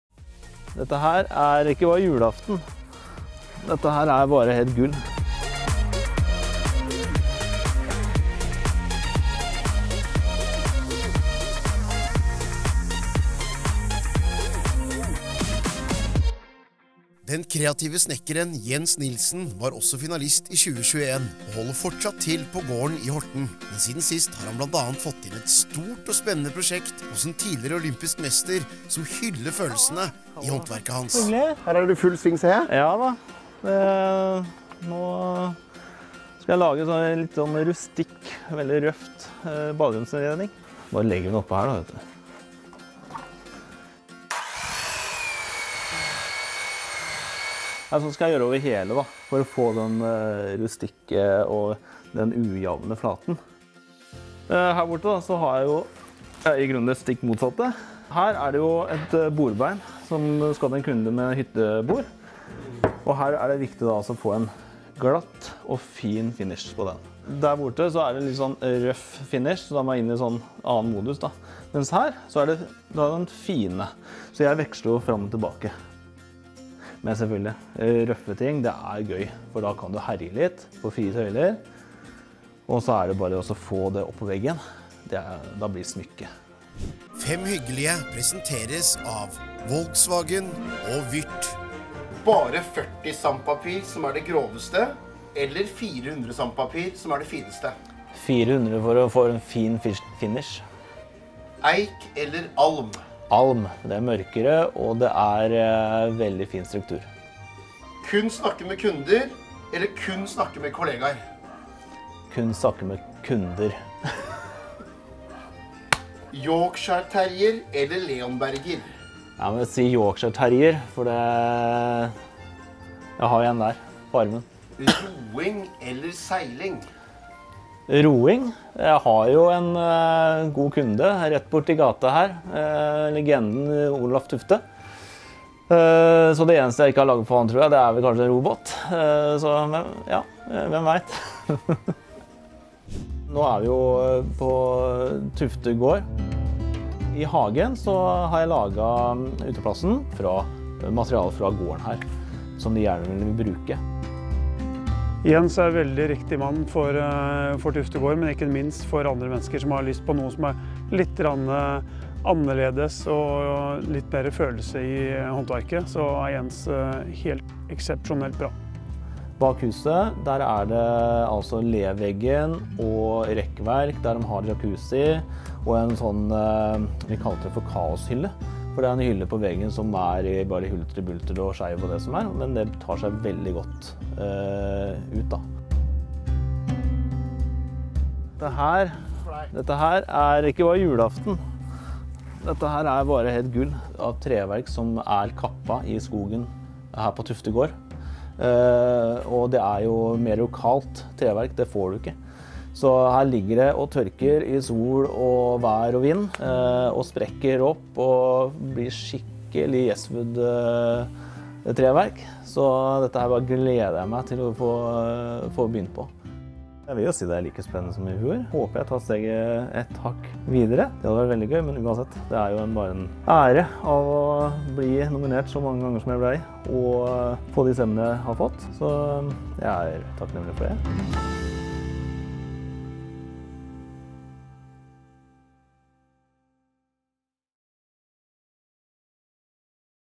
Norges Hyggeligste Håndverker 2022: Jens Nilsen
Det er ingen tvil om at interiør- og møbelsnekker Jens Nilssen er opptatt av trær. Han skaper flotte ting av dem, han nyter dem i naturen og han har til og med trær tatovert på ryggen! Allerede som 19-åring laget han sine egne møbler, men jobbet som plate- og sveisearbeider, før det en dag ble bråstopp: Hukommelsen forsvant, men han overlevde.